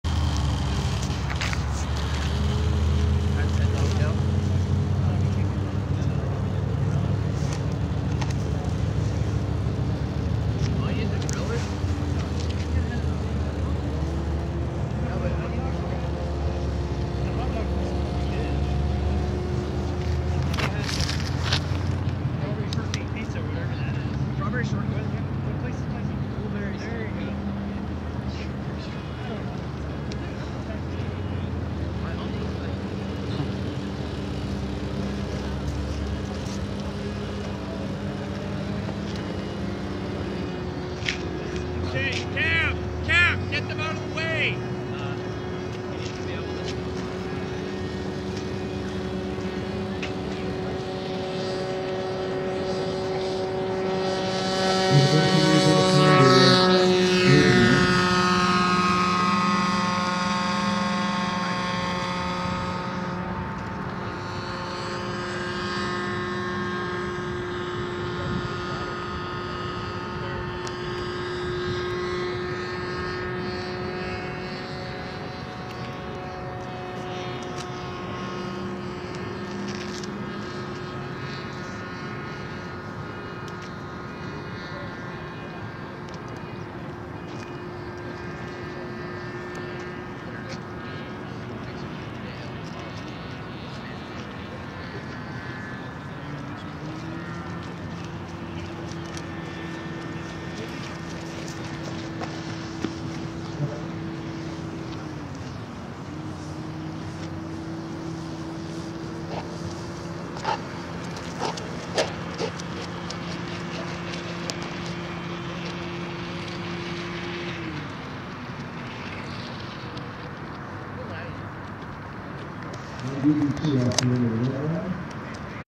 UBC AeroDesign 2010 - Competition Flight 3 - Afternoon and Rainy Conditions
Successful flight despite the continuing rain in the afternoon.